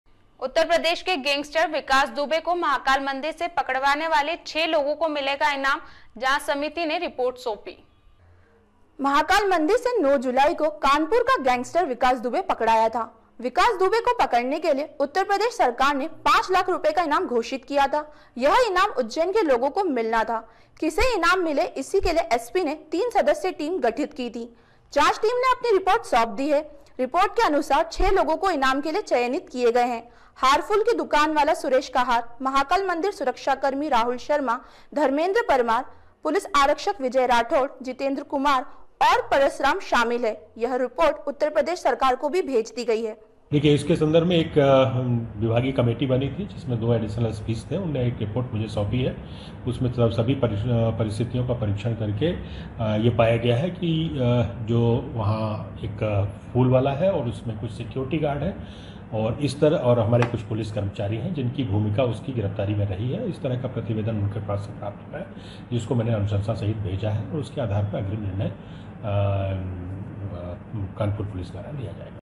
गैंगस्टर विकास दुबे को महाकाल मंदिर से पकड़वाने वाले 6 लोगों को मिलेगा इनाम
उत्तरप्रदेश के गैंगस्टर विकास दुबे को महाकाल मंदिर से पकड़वाने वाले 6 लोगों को मिलेगा इनाम, जांच समिति ने सौंपी रिपोर्ट,

 महाकाल मंदिर से 9 जुलाई को कानपुर का गैंगस्टर विकास दुबे पकड़ाया था विकास दुबे को पकड़ने के लिए उत्तर प्रदेश सरकार ने 5 लाख रुपए का ईनाम घोषित किया था यह ईनाम उज्जैन के लोगो को मिलना था किसे ईनाम मिले इसके लिए एसपी ने 3 सदस्य टीम गठित की थी जांच टीम ने अपनी रिपोर्ट सौंप दी है। रिपोर्ट के अनुसार 6 लोग इनाम के लिए चयनित किए गए है। हार फूल की दुकान वाला सुरेश कहार महाकाल मंदिर सुरक्षाकर्मी राहुल शर्मा, धर्मेंद्र परमार, पुलिस आरक्षक विजय राठौर, जितेंद्र कुमार और परसराम शामिल है यह रोपोर्ट उत्तर प्रदेश सरकार को भी भेजी गई है